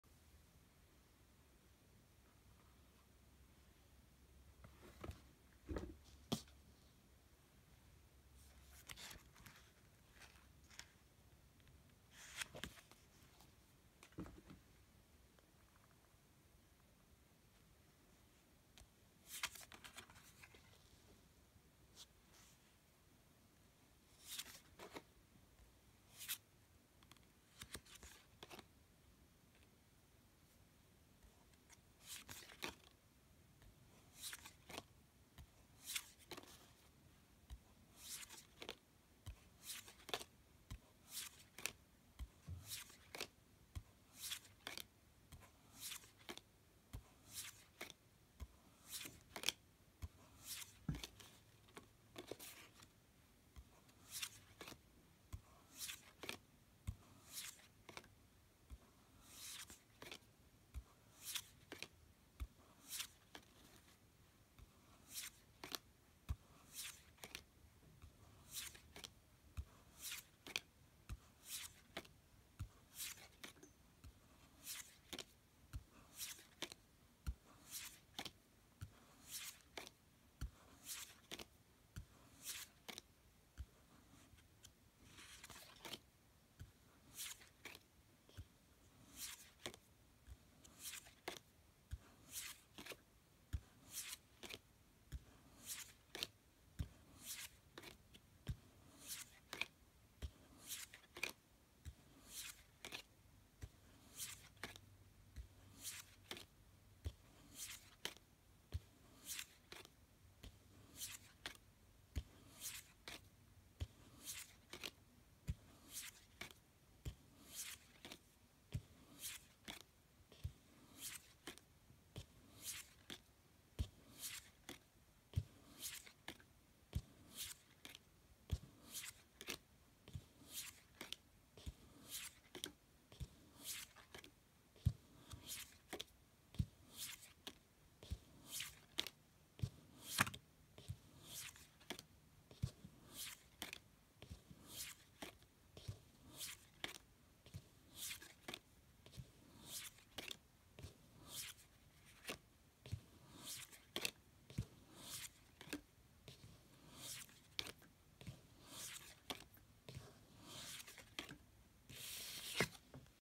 The Linestrider Tarot | Flip Through
A flip through of the Linestrider Tarot by Siolo Thompson.